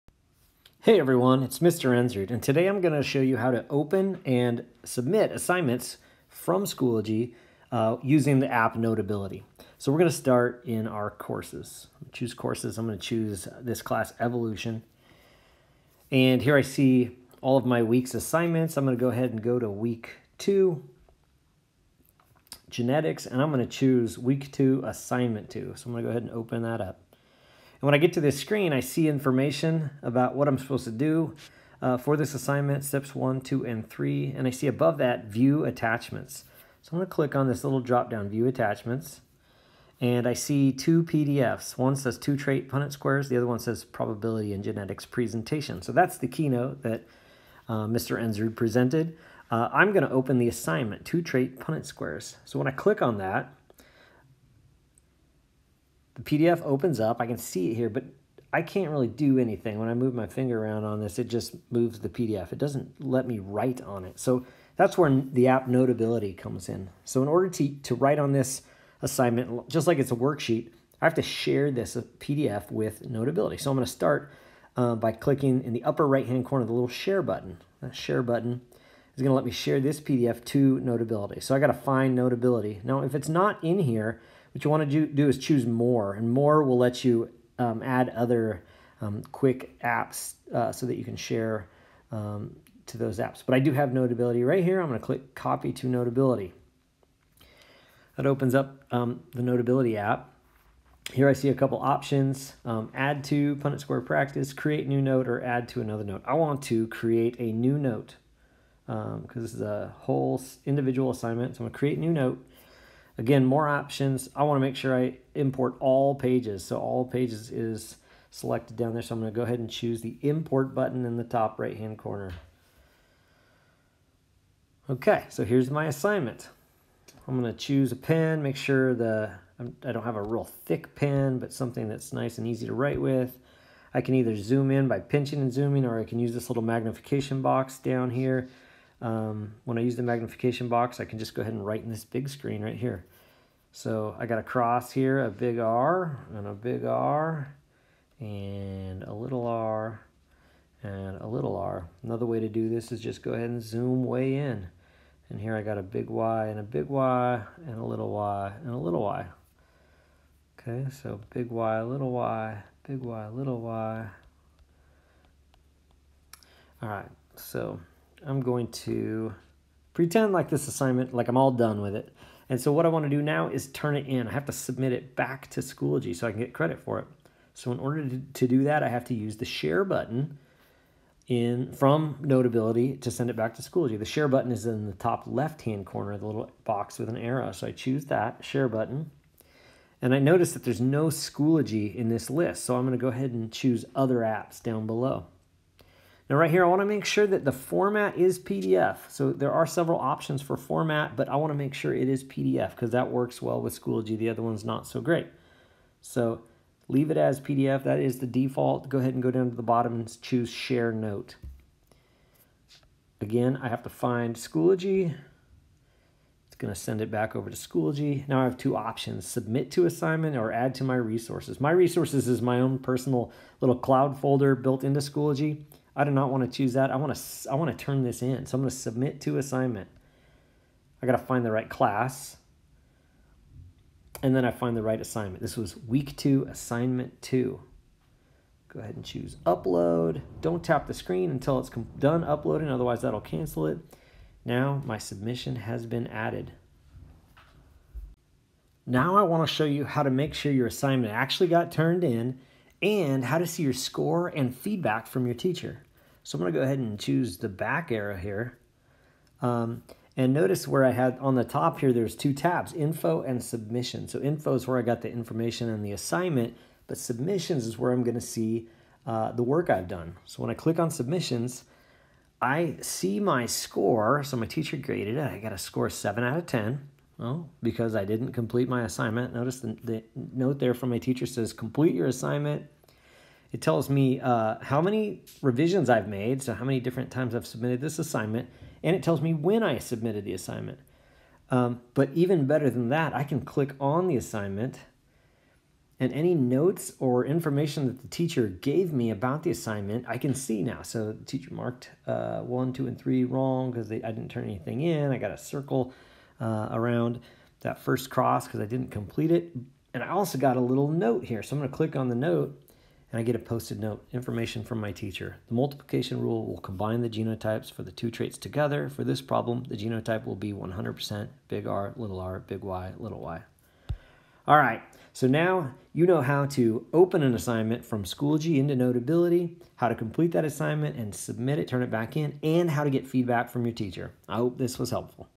How to Submit Schoology Assignments with Notability
This video explains how to open, edit, and submit assignments in Schoology using the Notability app on an iPad.